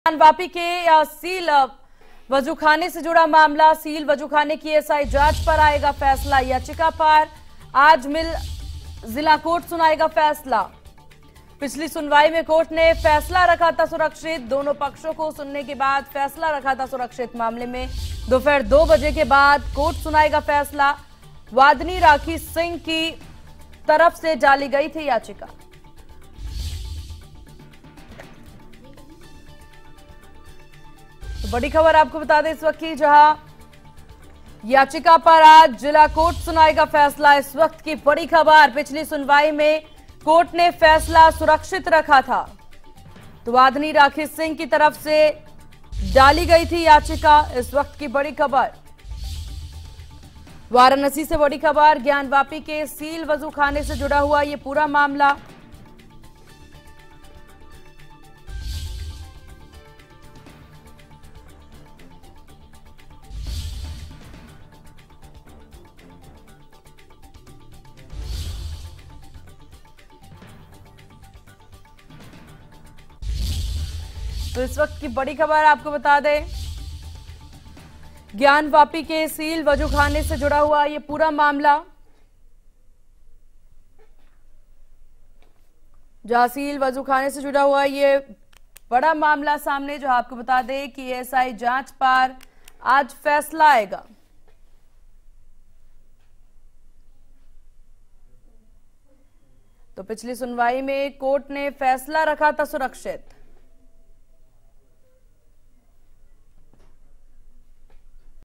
Gyanvapi Case:सील वजूखाने की ASI जांच पर आएगा फैसला, पिछली सुनवाई में कोर्ट ने फैसला रखा था सुरक्षित
Gyanvapi Case: सील वजूखाने की ASI जांच पर आएगा फैसला, पिछली सुनवाई में कोर्ट ने फैसला रखा था सुरक्षित | India News